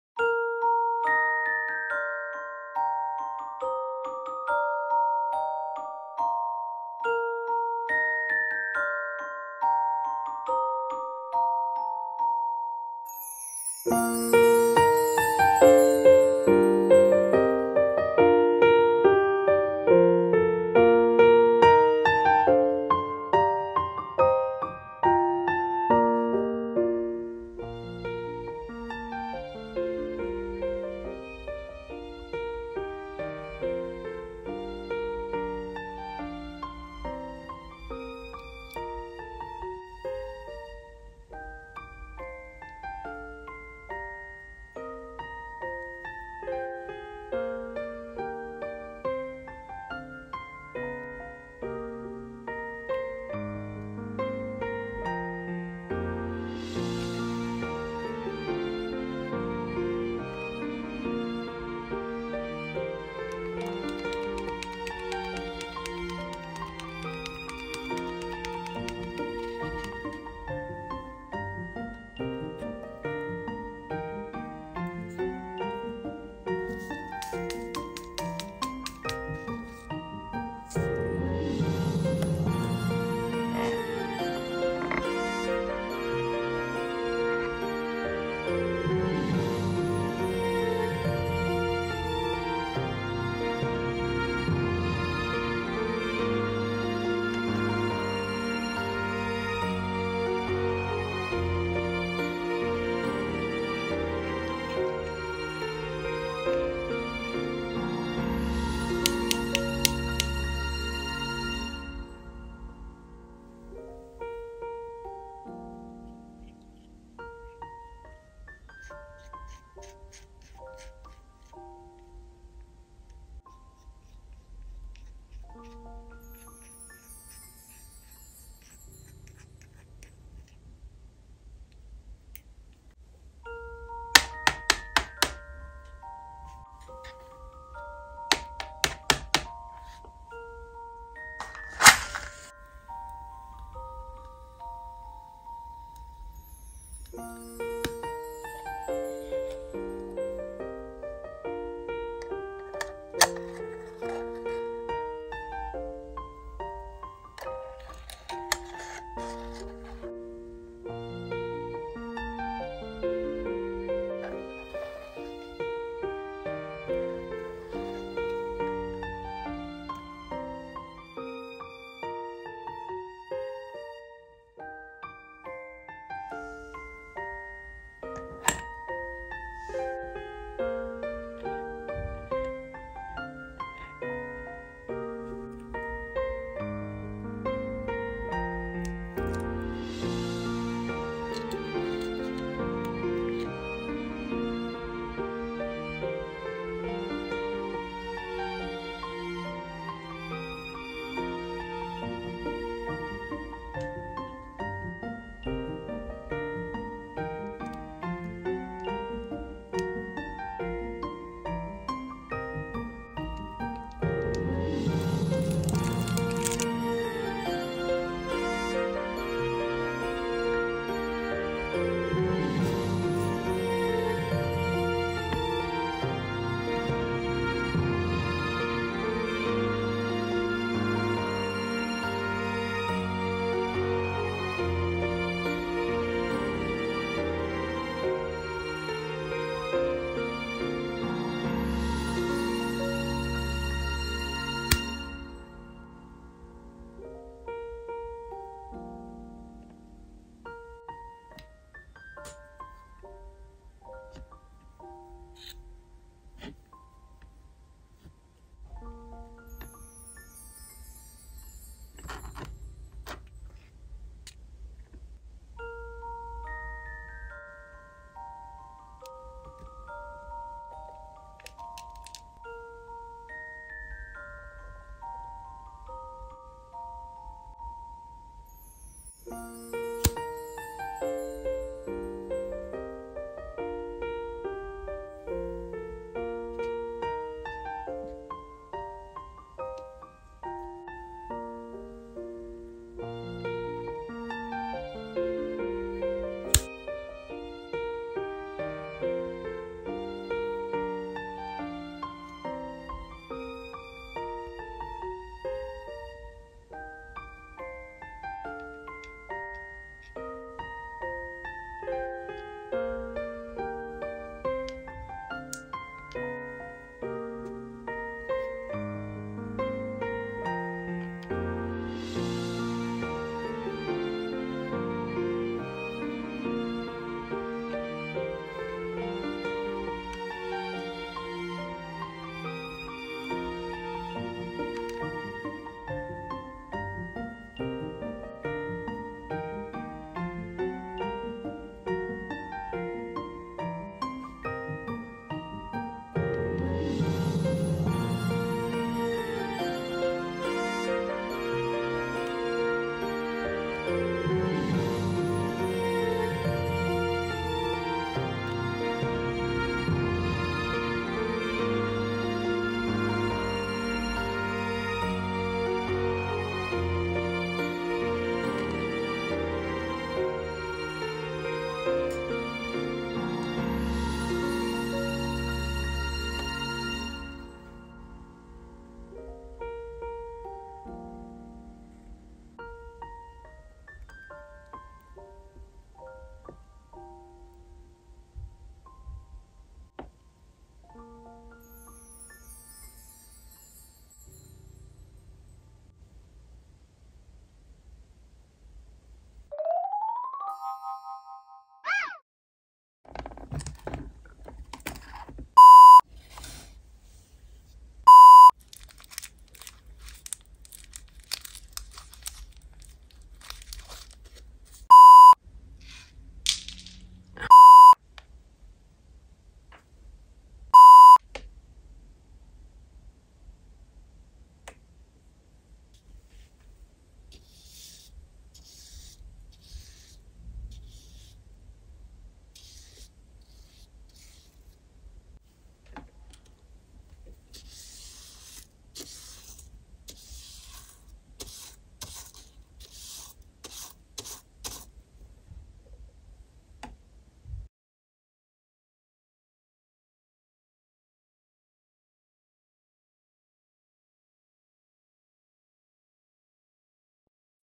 Miniature Rainbow Sky Cake! 🎂 Real edible Mini food cooking baking birthday tiny cake | minibuncafe
a real miniature rainbow sky birthday cake with handmade edible fondant details 🍰🎂 baked and decorated in the Minibun Cafe! Hope you enjoyed this video! if you did, let me know in the comnents 🥰 as always watch to the end for bloopers 🎬🤣

About minibuncafe: mini food, tiny cakes and other beautiful little things ❤️